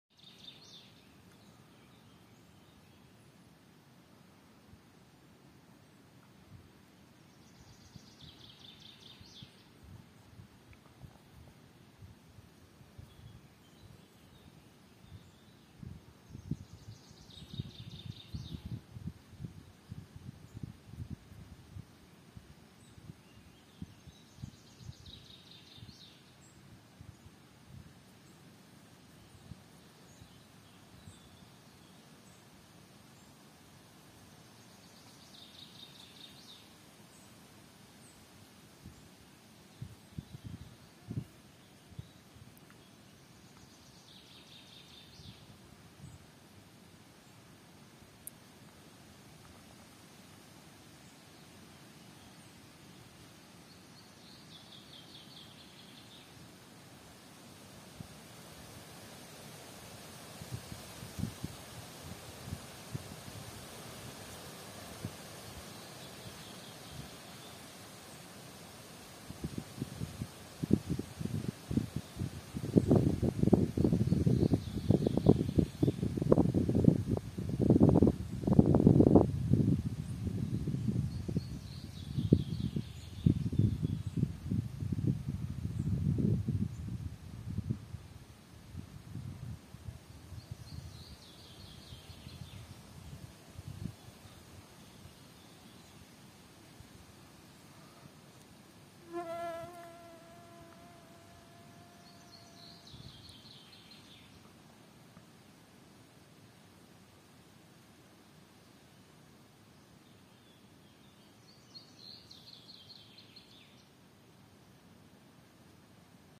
Funghi Porcini.Una raccolta dei funghi più belli raccolti nel 2023.Bellissimi i funghi porcini 2023
Un'altra splendida giornata nella foresta. Ti invito ad iscriverti, mettere mi piace e lasciare un commento. Un saluto a tutti coloro che amano la natura